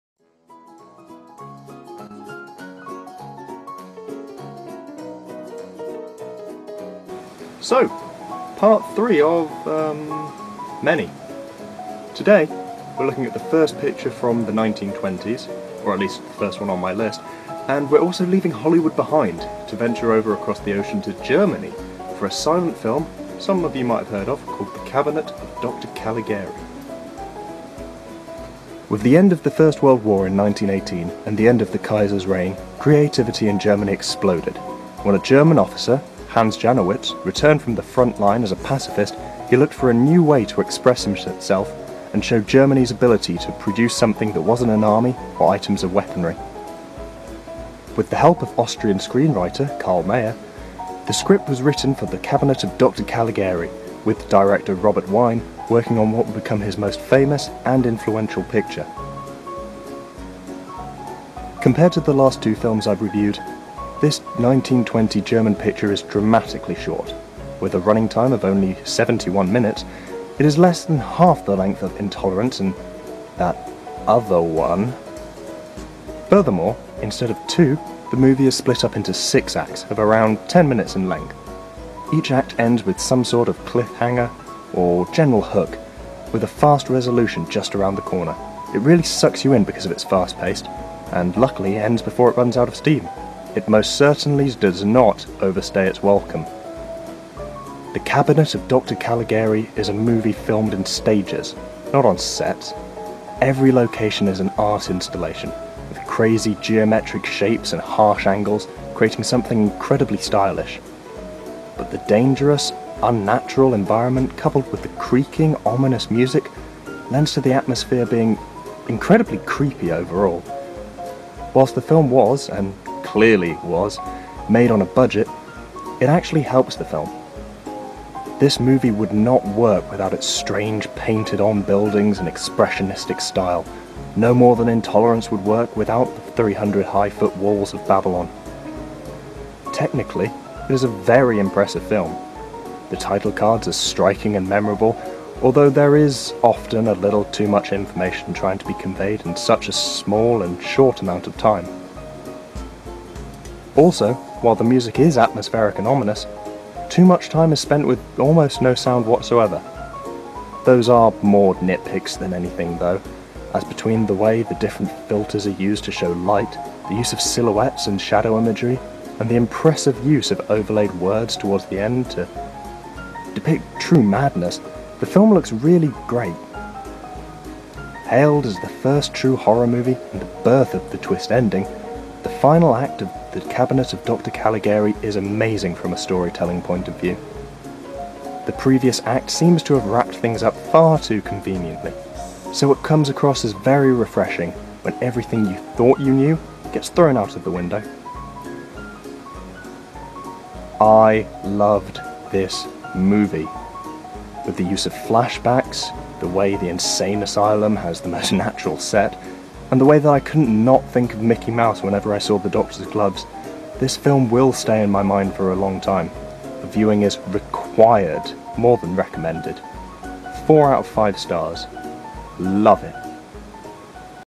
The Cabinet Of Dr Caligari (1920) Review
And so, on to part three of this film journey. Today, its 'The Cabinet of Dr. Caligari' - a 1920 silent movie from Germany.
If you like this video or any others in the series, please, spread the word. :)